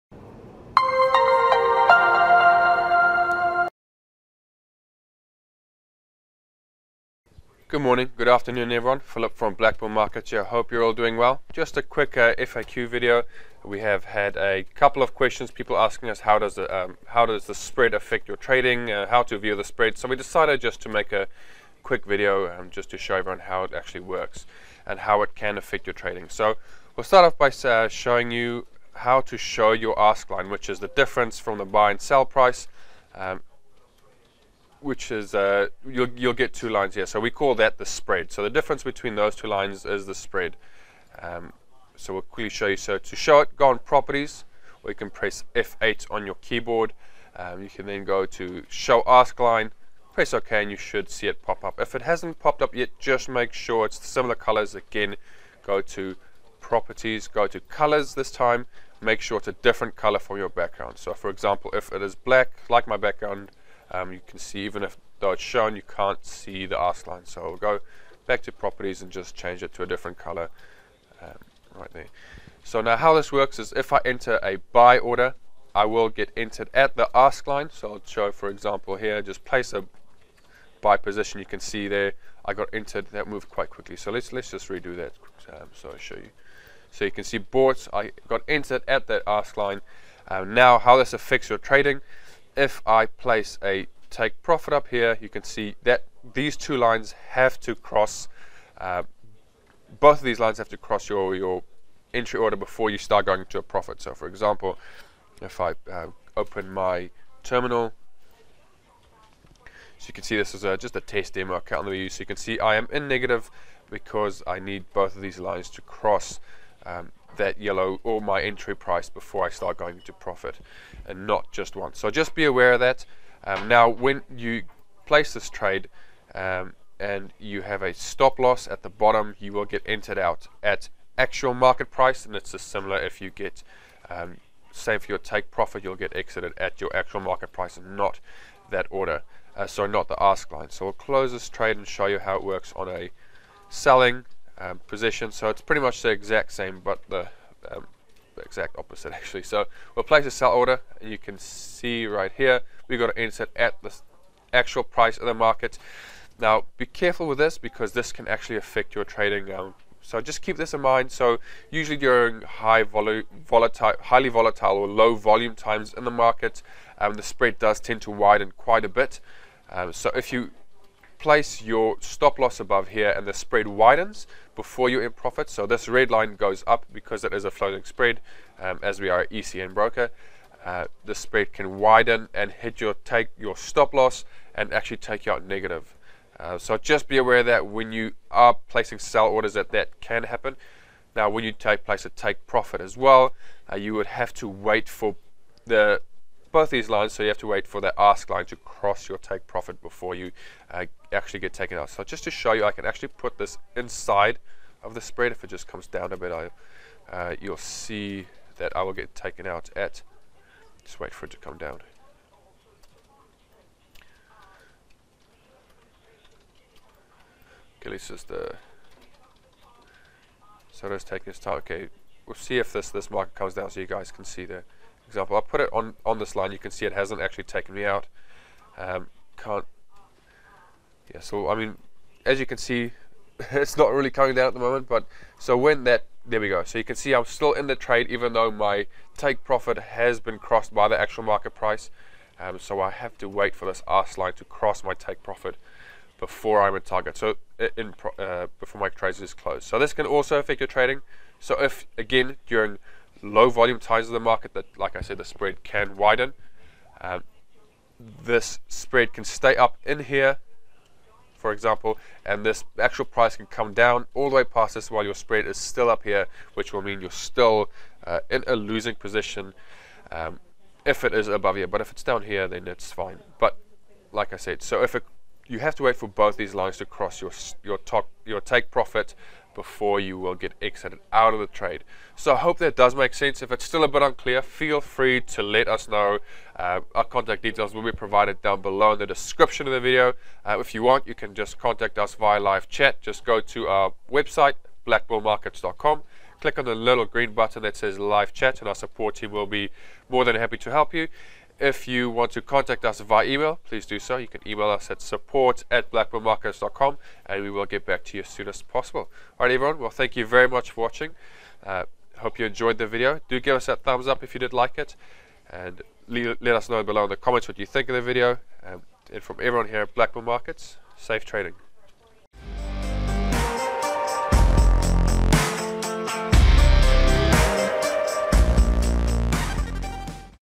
What is the Spread in Forex? | BlackBull Markets
● In this video, Philip breaks down what a spread is and how it works for your trading in MT4.
● Trade forex on BlackBull Markets.